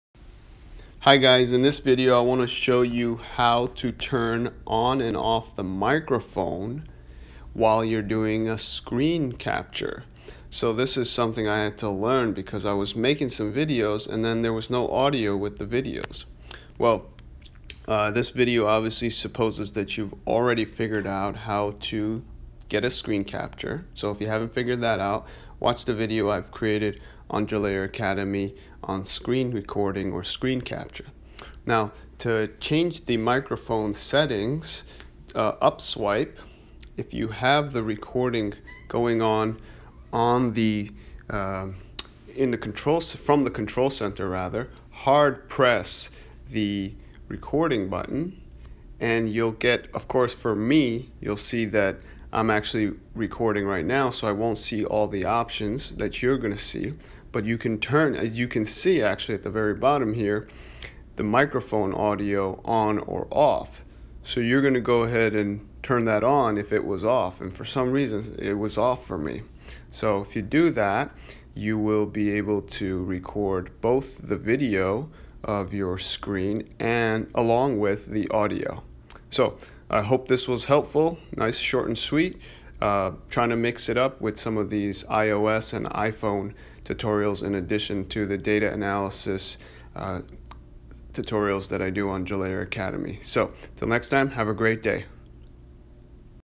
iPhone - turn Audio On or Off while Screen Capturing
Series on iPhone tips and tricks from Jalayer Academy.